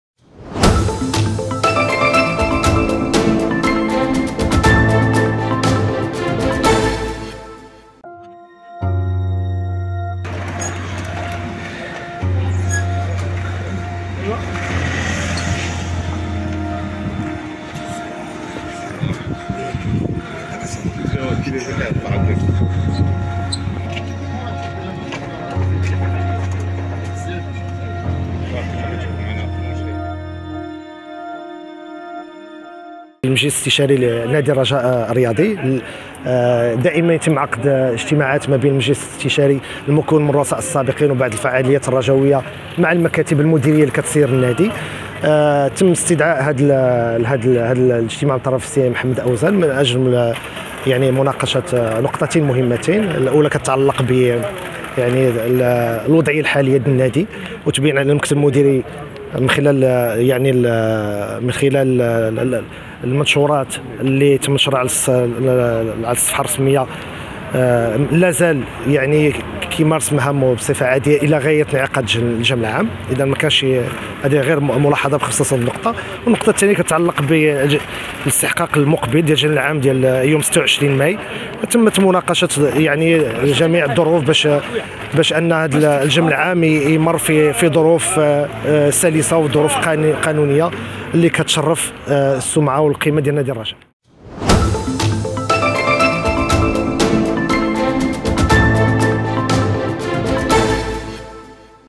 حكماء الرجاء الرياضي يجتمعون بمركب الوازيس في غياب البدراوي
اجتمع حكماء الرجاء الرياضي، الثلاثاء 9 ماي 2023، بمقر الفريق بالوازيس بالدارالبيضاء، من أجل تدارس الوضعية التي يمر منها في الوقت الحالي الفريق الأخضر، في غياب الرئيس عبد العزيز البدراوي، الذي دعا إلى عقد جمع عادي واستثنائي، من المقرر أن يقدم خلاله المكتب المديري الحالي استقالته، وبالتالي انتخاب مكتب مديري برئيس جديد.